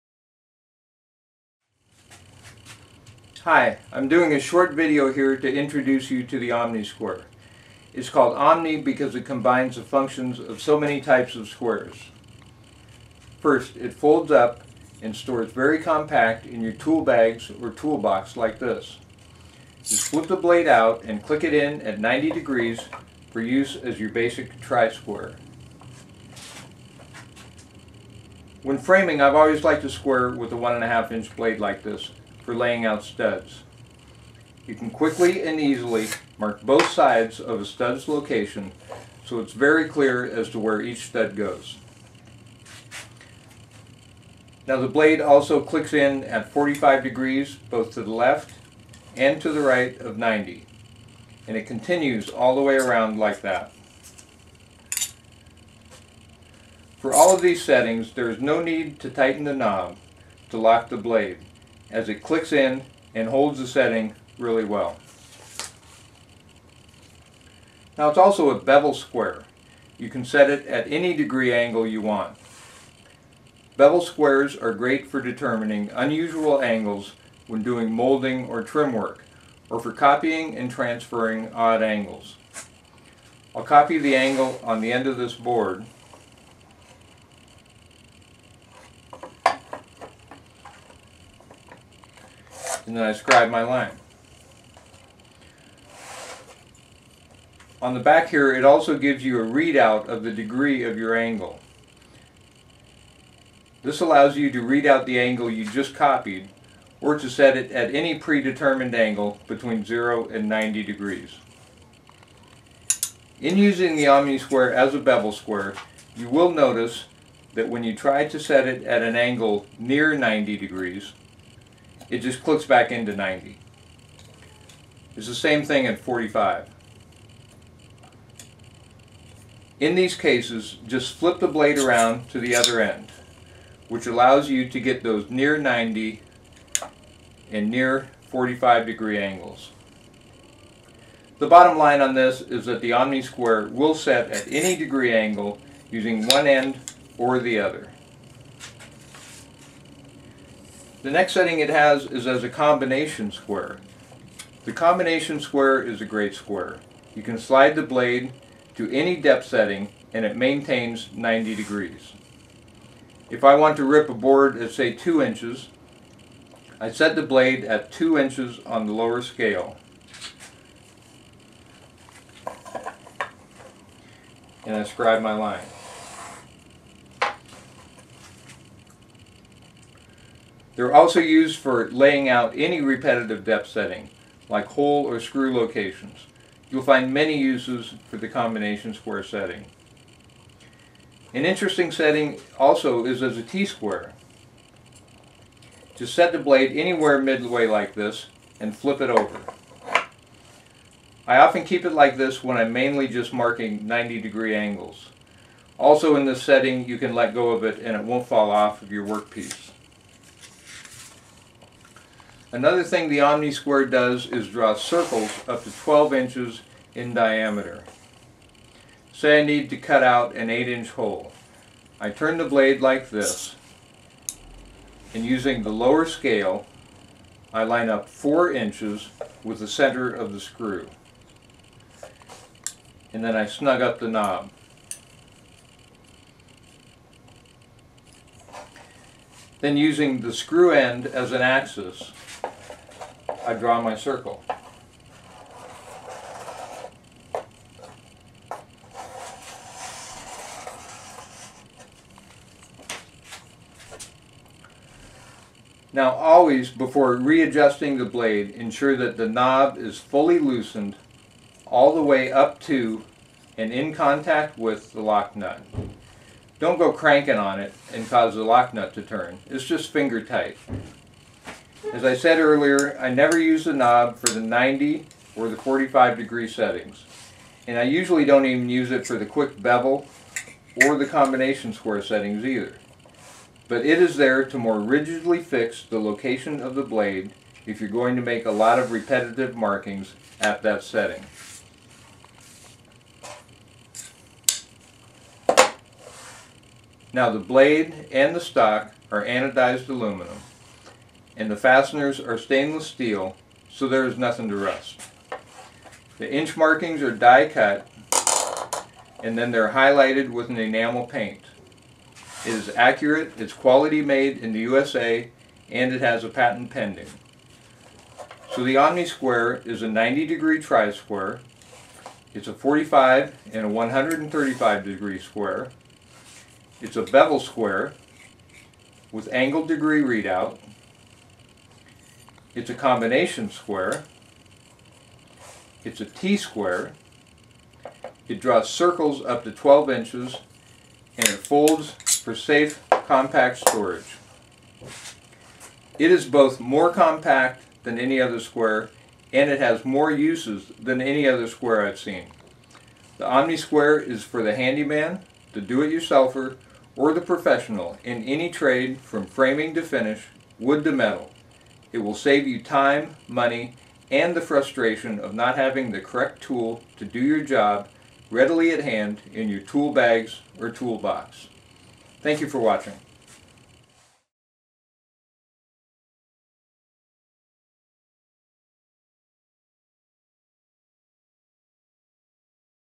T Bevel Combination Try Square in one
OmniSquare is a revolutionary, compact tool that does the work of several purpose-built tools: 90-degree Try Square, 45-degree Square, 135-degree Square, Bevel Square, Combination Square, T Square, and Circle Scribe. Designed and fabricated in America, the OmniSquare is made of industrial-grade anodized aluminum with  stainless steel hardware and now with deep laser cut markings. The OmniSquare can be configured quickly and folds up for safe, compact storage. OmniSquare is the perfect tool for a carpenter, handyman, handywoman, framer, metal worker, hobbyist, woodworker, or for any do-it-yourself projects. Also makes a great gift for any of the above!